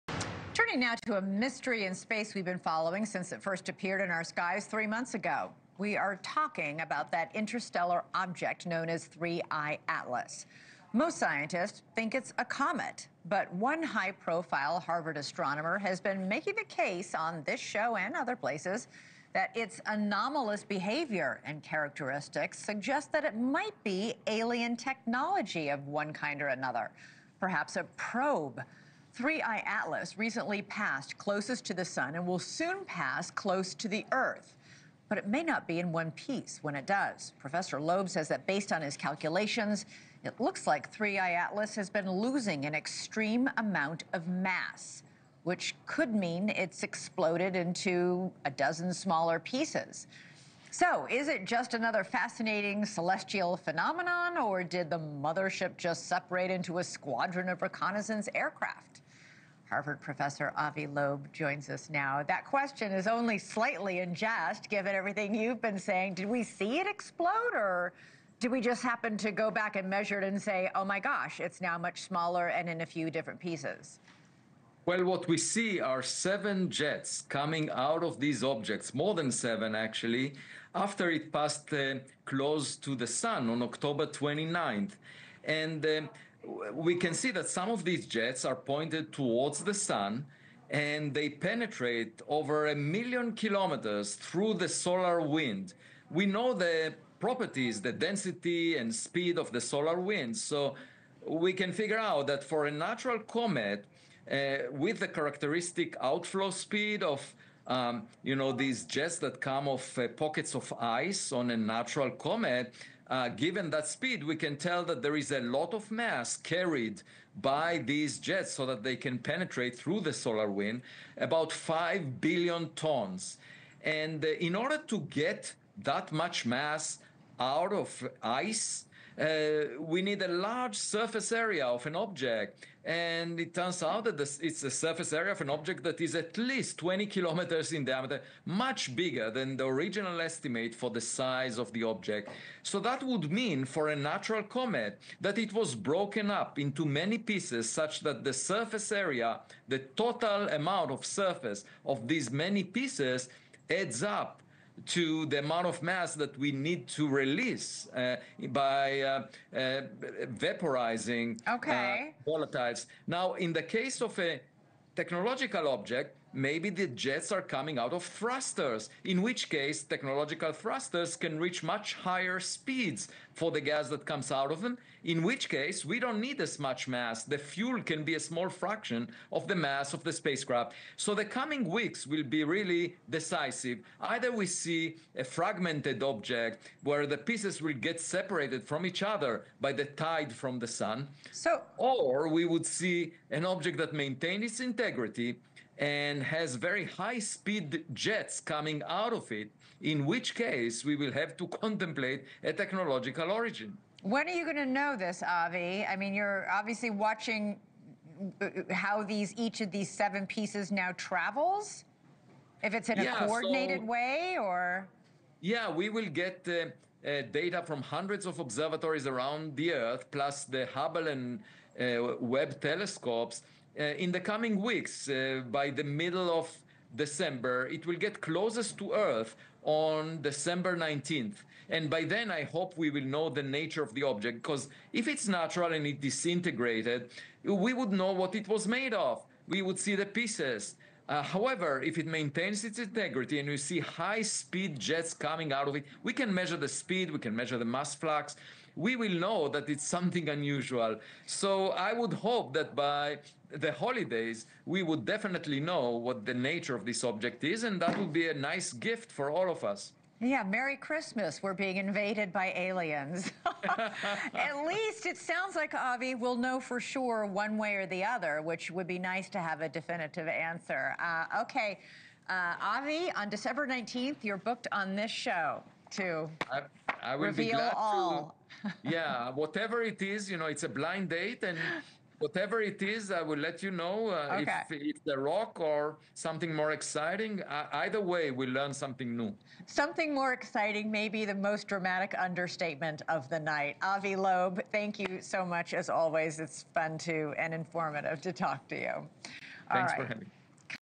阿维勒布：3I/ATLAS 解體｜七道噴流、質量消失！外星探測器？｜Avi Loeb: 3I/ATLAS Breaking Apart, Suggesting Alien Probe？
哈佛大學天體物理學家 阿維·勒布在今天（11月10日）的訪談——他指出星際天體 3I/ATLAS 的行為異常：
七道高速噴流、數十億噸質量消失、可能分裂成多個碎片。
這究竟是一顆自然的彗星？還是外星文明留下的「探測器」？
他預告：12月19日 將是揭曉真相的關鍵日！
這場對話融合了科學推理與宇宙想像，帶你深入太陽系邊界的未知。

Harvard astrophysicist Avi Loeb joins to discuss the mysterious interstellar object 3I/ATLAS — now showing seven jets, losing billions of tons of mass, and possibly breaking apart.
Is it just a natural comet, or could it be alien technology?
Loeb predicts that by December 19, we may finally know the truth.
A thrilling conversation that blends cutting-edge science with cosmic wonder.